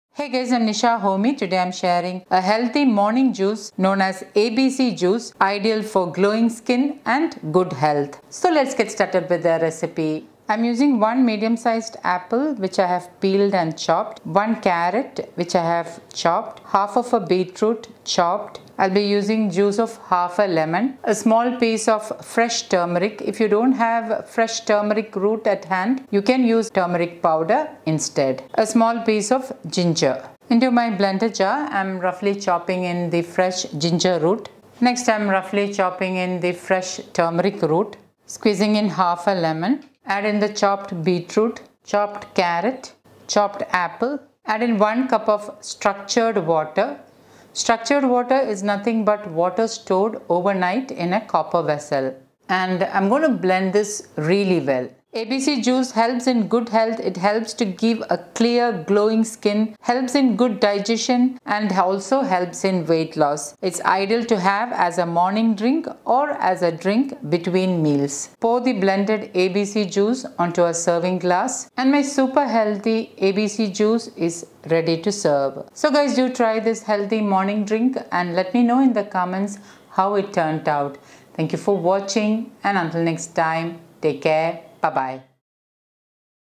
ABC Juice - Healthy Morning Juice For Good Health & Skin Care - ABC Juice Recipe Healthy Weight Loss
today, I am sharing the recipe of miracle drink known as ABC juice with three main ingredients - Apple, Beetroot, Carrot along with turmeric and ginger. This healthy morning juice is rich in vitamins, antioxidants and fibre. It helps to remove toxins from the body and give a glow to your skin. The other benefits of ABC juice are it reduces the high blood pressure, increases immunity and in easy digestion. It is also ideal to include in weight loss diet.

how to make abc juice, healthy morning juice for weight loss, healthy skin care, miracle drink recipe.

Ingredients:
1 apple
1/2 beetroot
1 carrot
1/2 inch ginger
1/2 inch turmeric root
1 cup structured water (water stored overnight in a copper vessel)
This recipe serves 2 glasses of juice.
Note - Ideal to have this juice 2-3 times a week.

Things I Love To Use:
Blender I Used
Apple Cider Vinegar
Moringa Powder

XoXo
Nisa